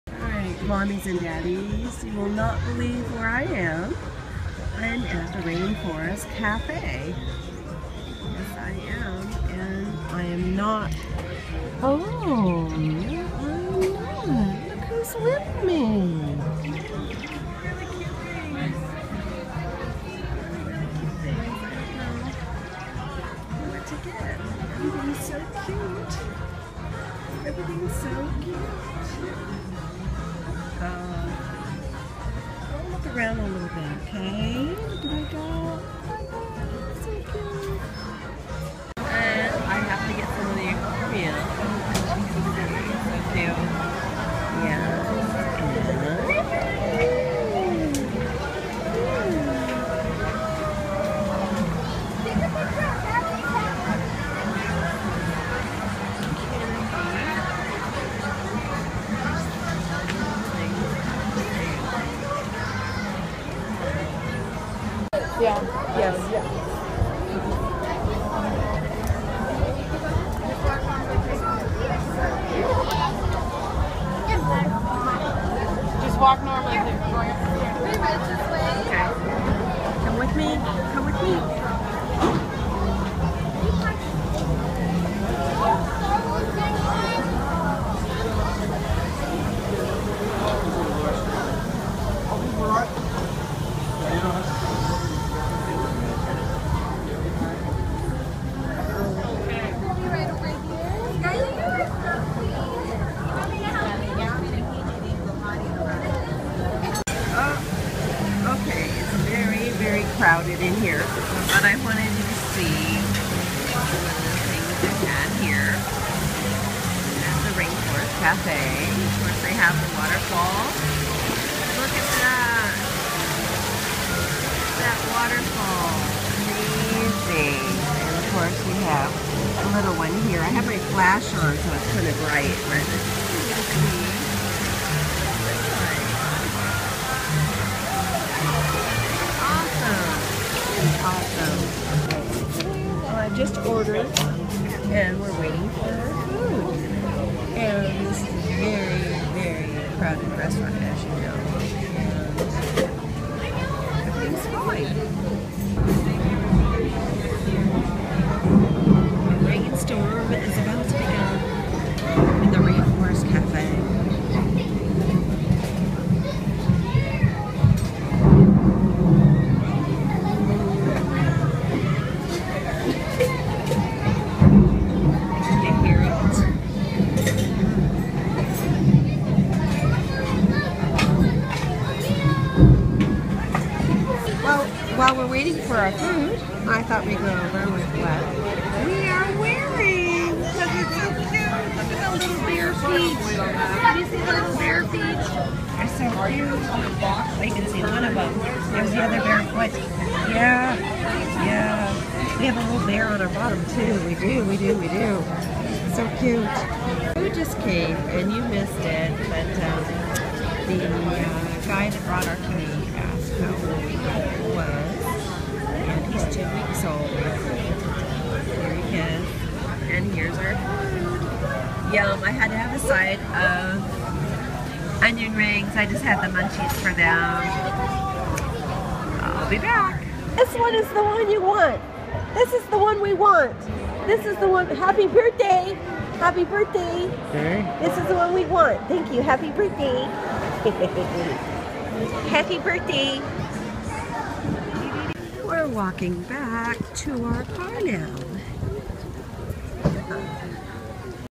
Reborn Baby Outing: Dinner at the Rainforest Cafe! "Noe sculpt by Wosnjuk"
In celebration of my Birthday, we went to Rainforest Café for dinner. I brought along Reborn Noel to a very crowded restaurant holding him in my arms the whole time. I have a terrible cold you can hear it in my voice. I was so sick I had a hard time keeping track of when I was recording and when I wasn't. Caught audio of me asking for a present with no picture. Funny!